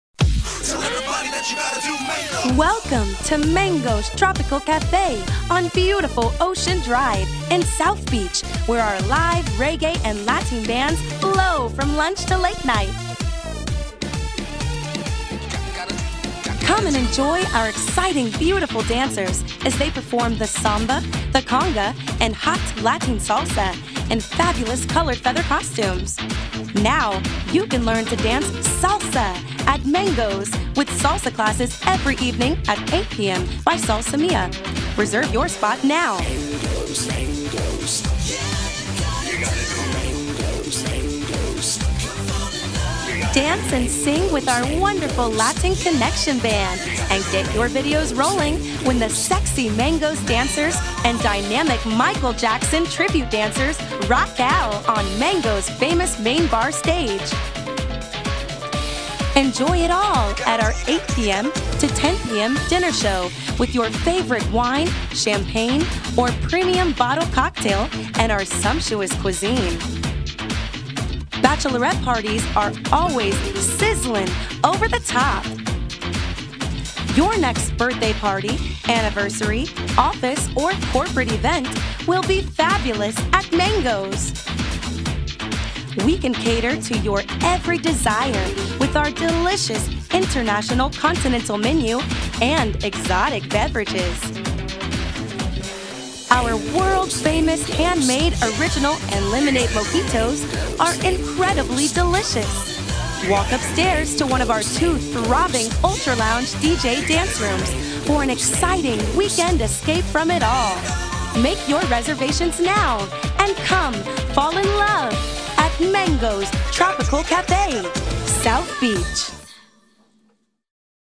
World Famous Mango's South Beach
Our amazing show begins nightly at 8:15 p.m., featuring live performances including Samba, Cuban Conga, the Michael Jackson Tribute, Salsa, Bachata, a mix of 80’s and 90’s music, and more. Paired with our Caribbean and Latin cuisine and you have yourself a perfect night.

Right on the famous Ocean Drive, we have been hosts to unforgettable parties for twenty-seven years with the perfect drinks and the best location. Join us in the world-famous Vodou Room Ultra Lounge at Mango’s South Beach.